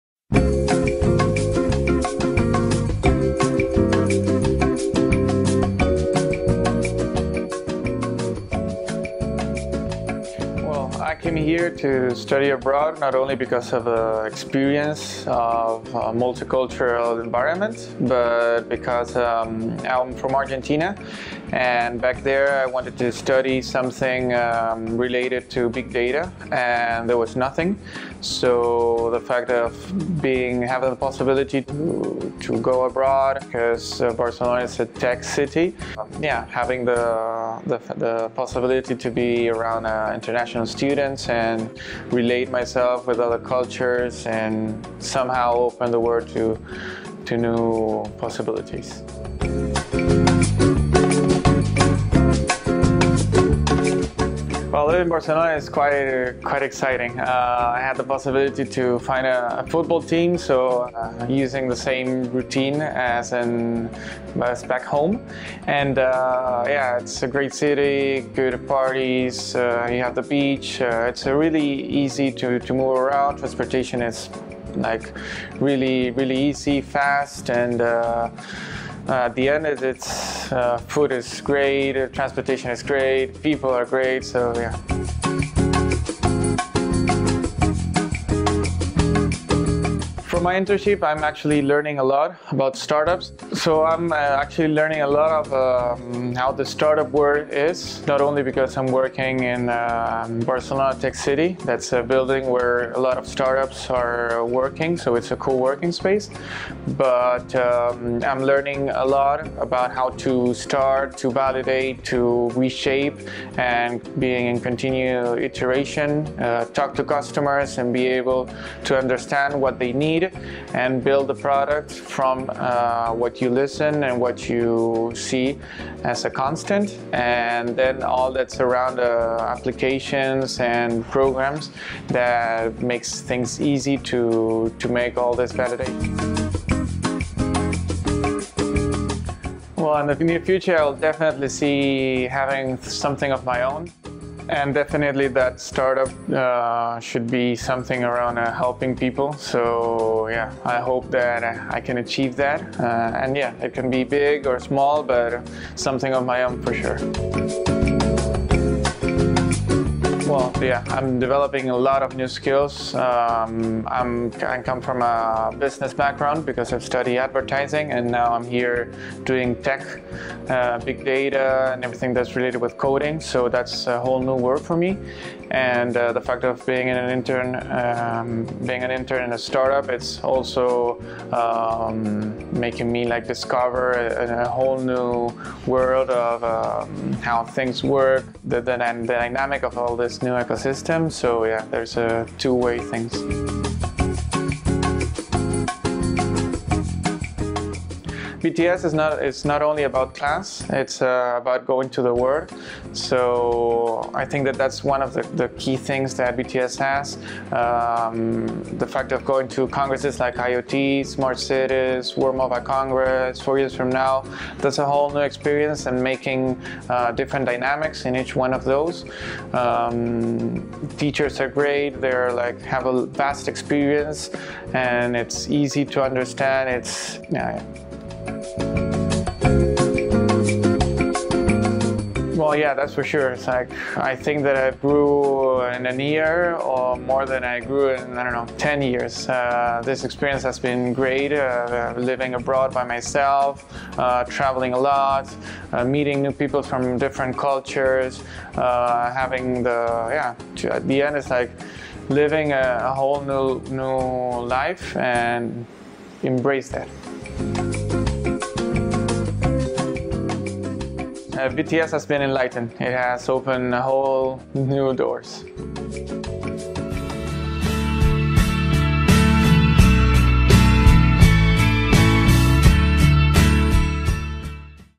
Barcelona Technology School Review by Franco Bonifetto
Franco Bonifetto is a student from the Master in Big Data Solutions at Barcelona Technology School. He comes from Argentina, and let's discover his new experience in the world of data in Barcelona.